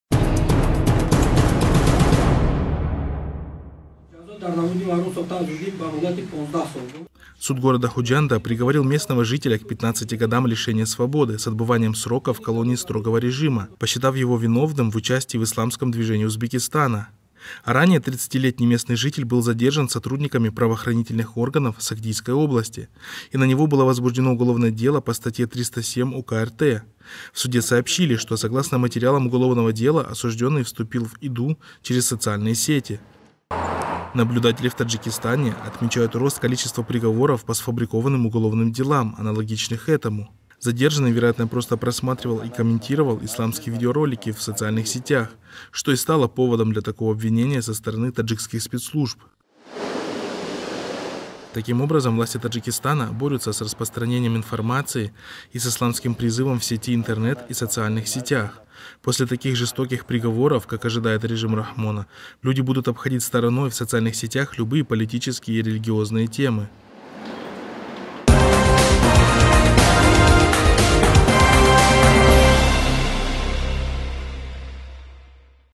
15 лет тюрьмы за социальные сети
Суд города Худжанда приговорил местного жителя к 15 годам лишения свободы с отбыванием срока в колонии строгого режима, посчитав его виновным в участия в “Исламсмком движении Узбекистана”.